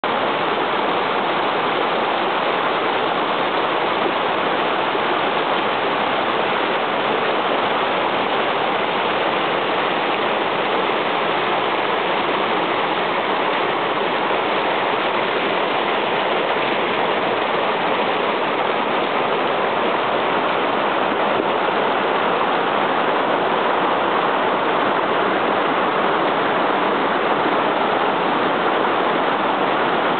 Mt. Tam Creekside after a rain, part 2
The sights and sounds of a redwood forest after a winter rain in Marin County, California. Mt. Tamalpias is where many say the sport of mountain biking got its start! It was a quick stop on my mountain bike ride there in Feb. 2007.